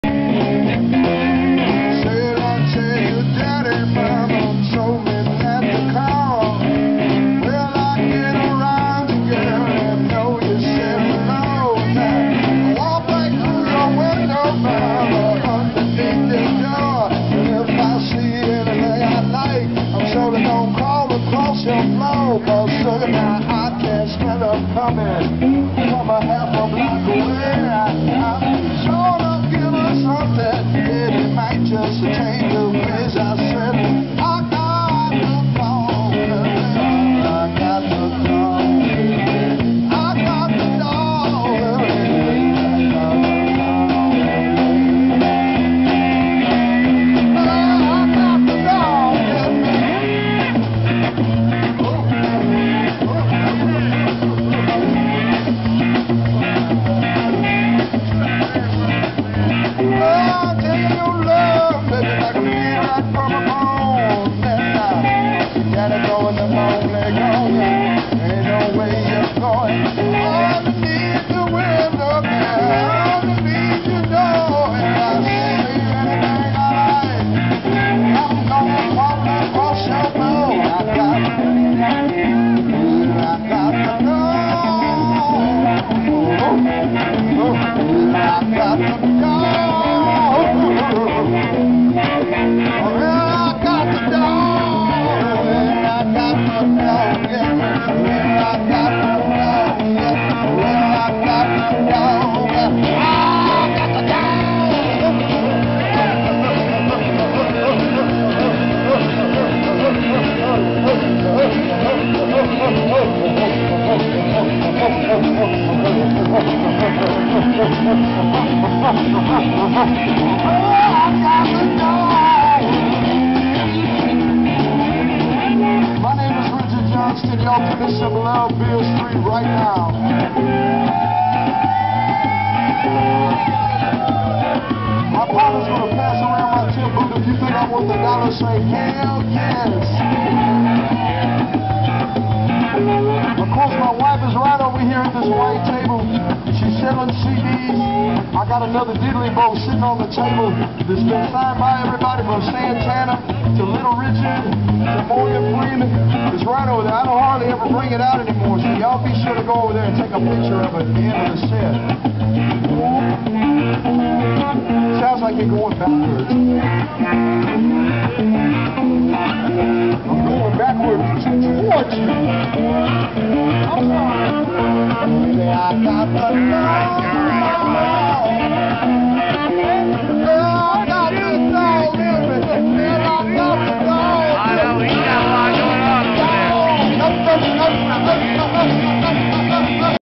Richard Johnston: Hill Country Troubadour - Beale St. 2009
The great Richard Johnston playing the Blues on Beale Street.  Great performer, and he plays for tips!  Some of the best entertainment you can find on Beale, free or otherwise!